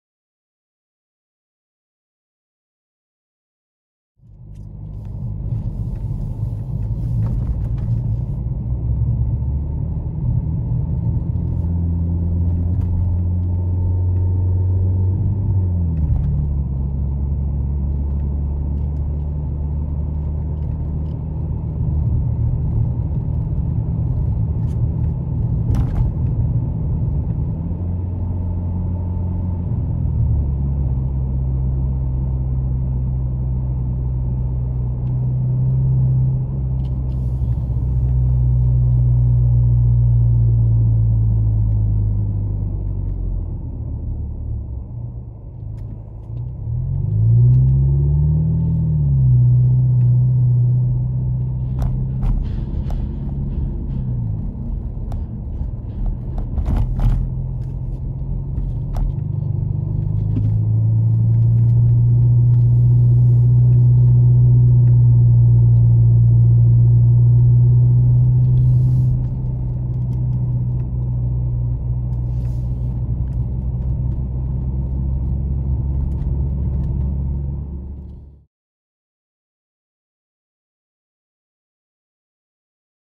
Snippets! Jaunt Through Jordan
Short clip of the longer video drive in Jordan ON